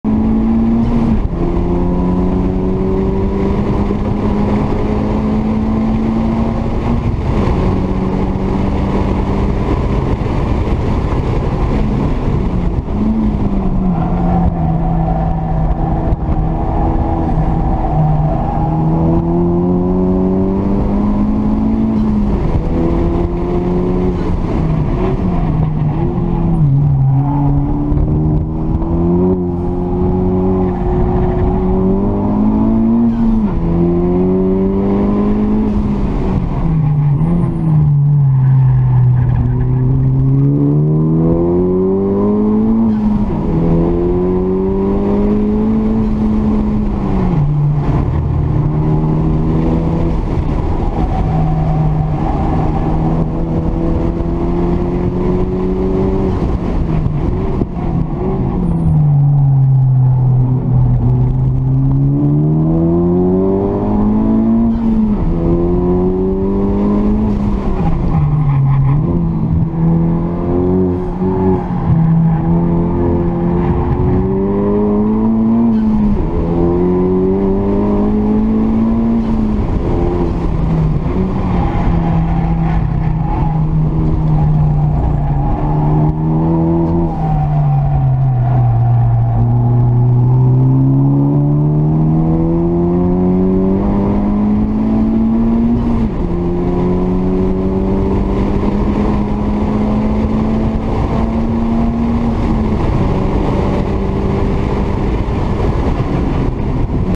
Civic type R FK8 @ Tazio Nuvolari race track
Personal best: 1.34.7 ,

Hot day at the race track , 34° C .
Car finally has been ecu tuned and decatted.
3 laps before experiencing overheating problems, other fk8s with different engine mods did not exceed 1.5 laps before having to lift off to cool down. Low performance Toyo r888r tires, good grip at the beginning but became understeer in the middle of the day. From the next track day I will try Nankang AR-1 tires.
There is still margin to improve, at least 1 second per lap, overall satisfied with the day.
Car is becoming competitive.